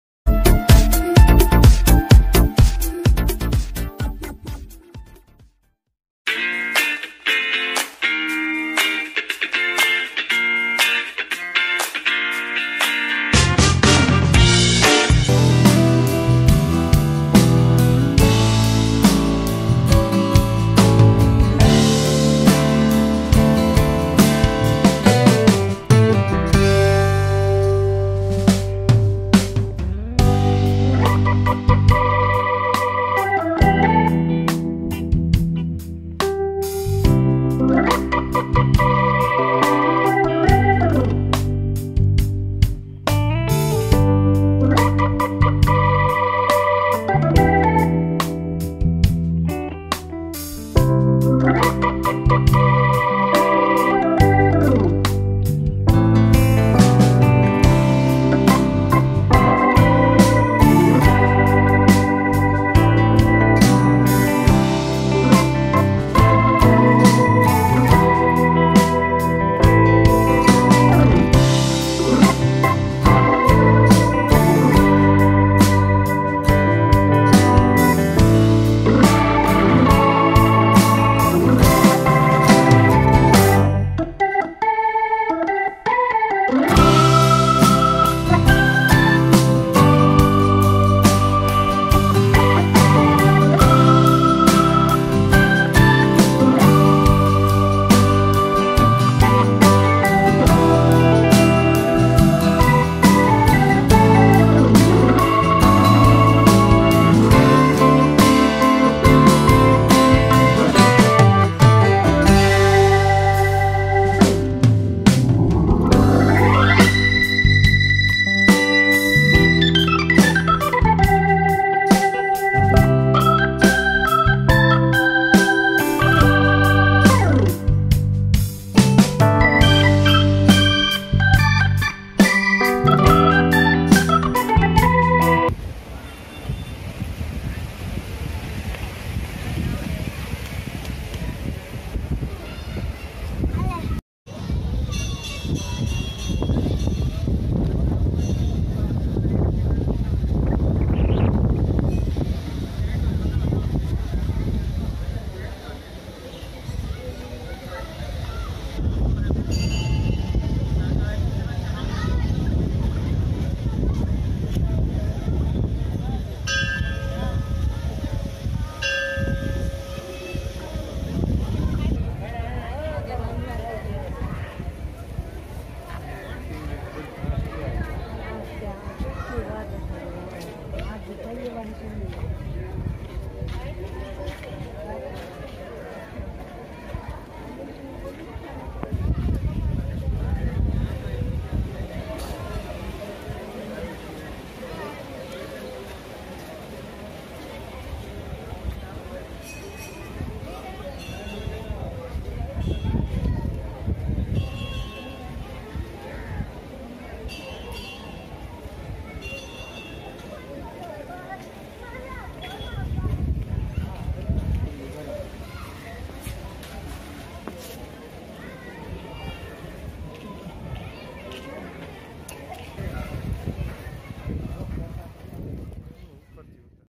siddhnath temple gujarat short video
this video about a historical shive temple  this temple name is siddhnath mahadev temple its nearyby my surat city